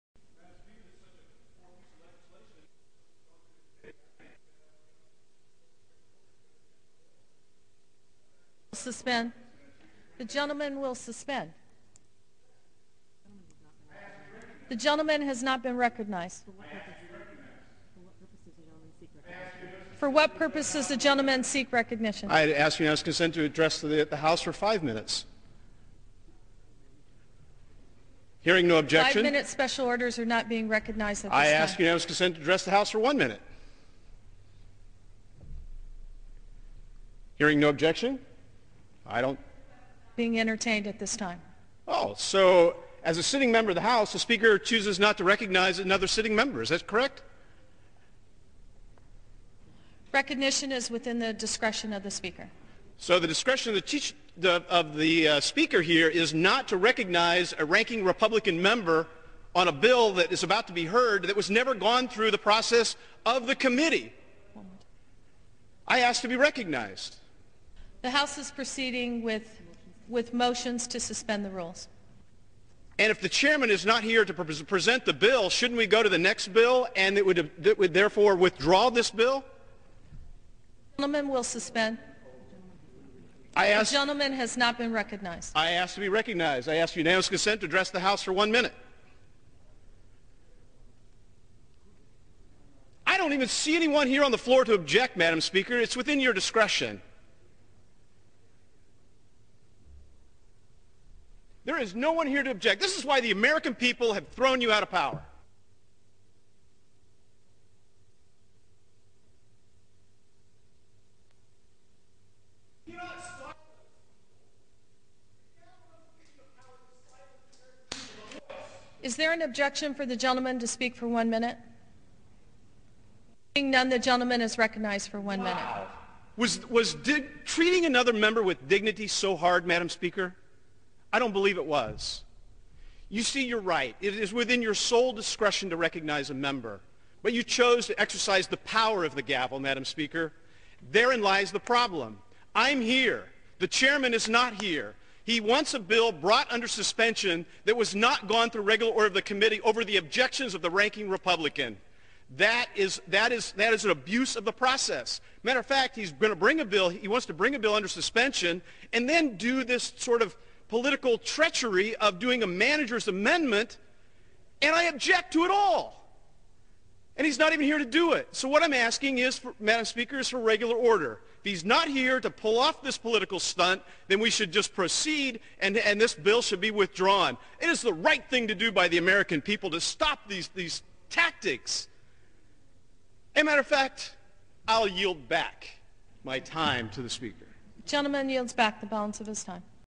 Rep. Steve Buyer Lambastes Dems After Refusing to Let Him Speak
"This is why the American people have thrown you out of power" -Rep. Steve Buyer (R-Indiana) to the Democrat who controlled the floor.

Thank you America for voting these statist thugs out of power!